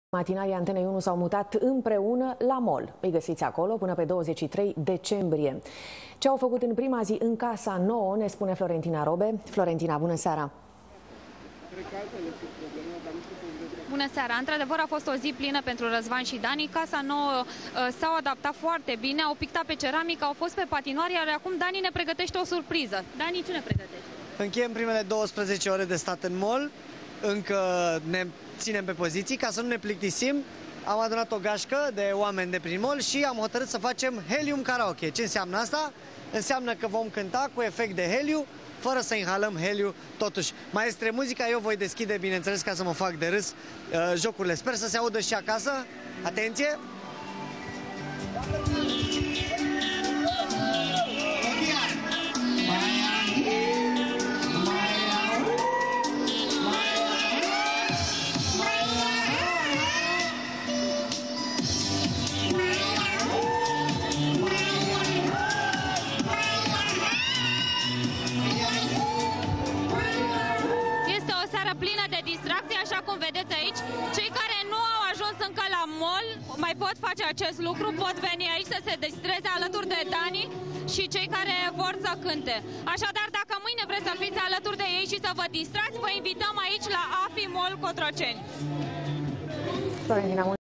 Razvan si Dani si-au facut o locuinta la mall 19 DECEMBRIE 2011
Razvan si Dani matinalii Antenei 1 s-au mutat impreuna. Si-au facut o locuinta la mall. Ii gasiti acolo pana pe 23 decembrie.